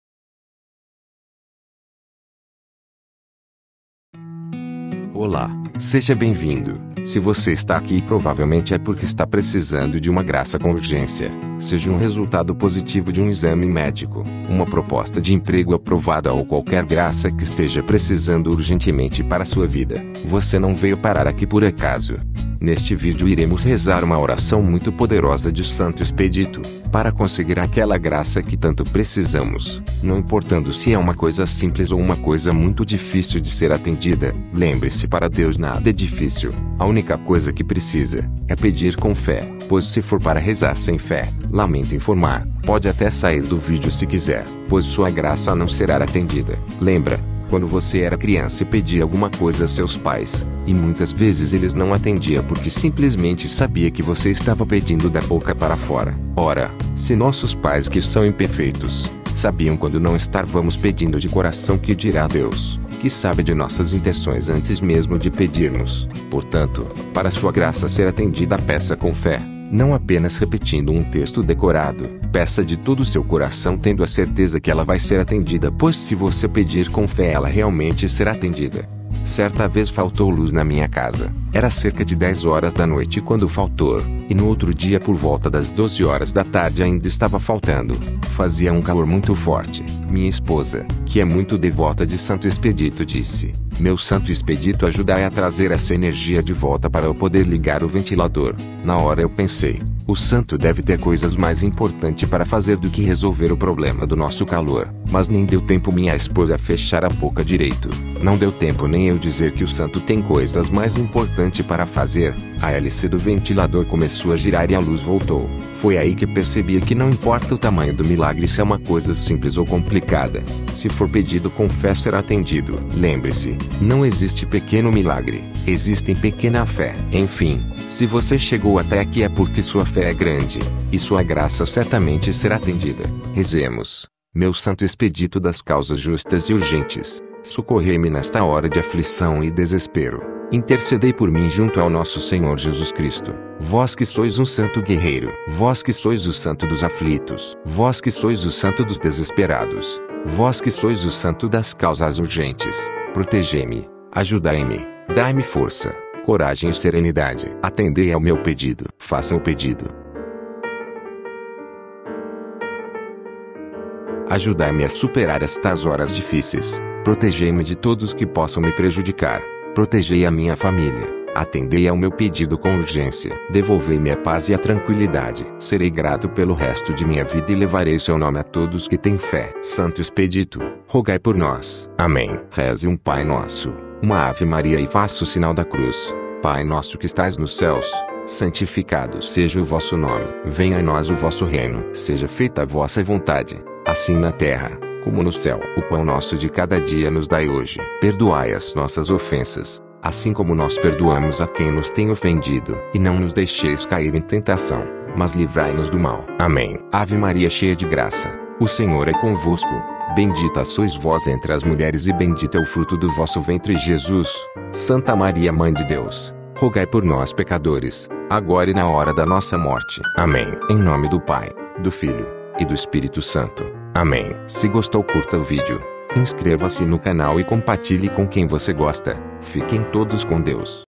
Seu milagre chegará hoje, Oração de santo Expedito.
Nesse vídeo, faremos uma poderosa oração a Santo Expedito, o santo das causas urgentes, para conseguirmos com urgência, aquela graça que tanto buscamos. Não importando se é uma coisa mais simples, como uma proposta de emprego, ou  algo muito complicado, como a cura para uma doença grave que os médicos já desenganaram. A Única coisa que precisamos é pedir com fé.

Lembre-se:
"Pedi e se vos dará. Buscai e achareis. Batei e vos será aberto.  Porque todo aquele que pede, recebe. Quem busca, acha. A quem bate, abrir-se-á. Quem dentre vós dará uma pedra a seu filho, se este lhe pedir pão?  E, se lhe pedir um peixe, dar-lhe-á uma serpente?  Se vós, pois, que sois maus, sabeis dar boas coisas a vossos filhos, quanto mais vosso Pai celeste dará boas coisas aos que lhe pedirem." Mateus 7, 7-11